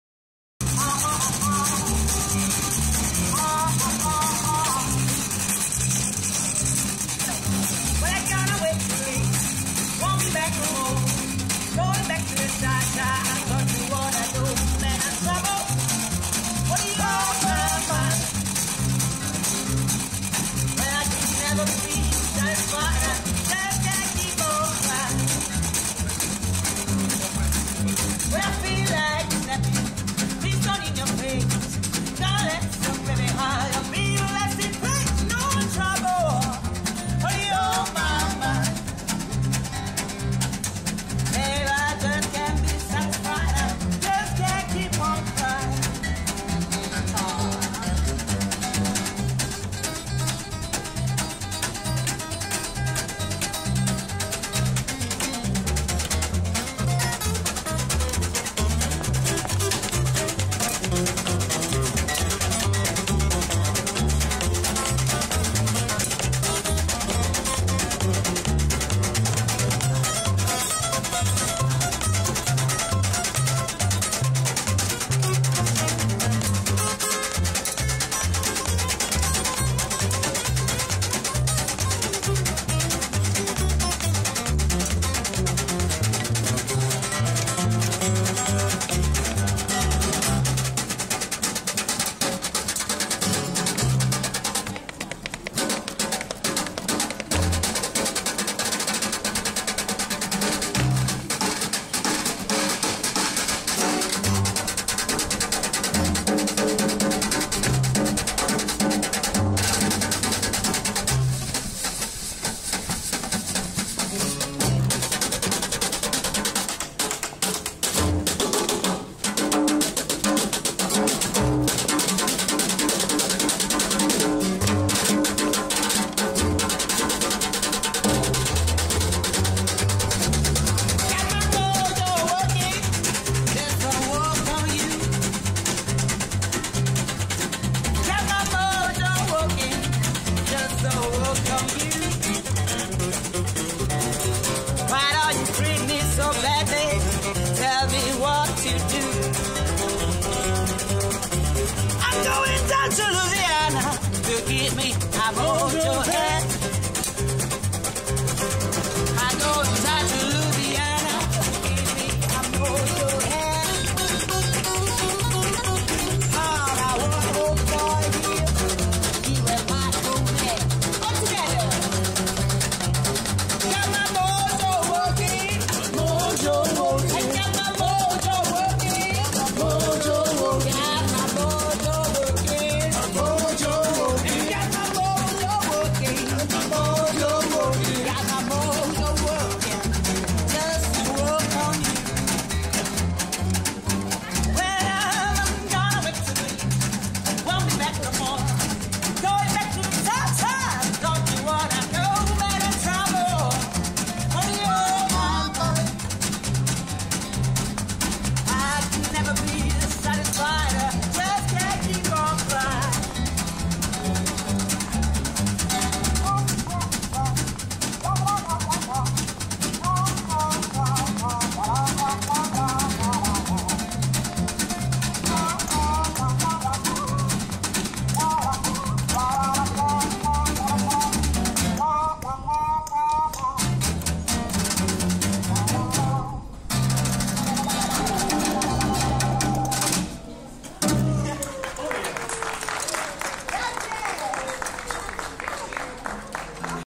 ALLIGATOR NAIL - Street Version video 2 - 2014
Stephanie Ghizzoni - voice, percussions, toys
Gigi Cerpelloni - guitar
Paolo Xeres - snare drum
Marco Xeres - upright bass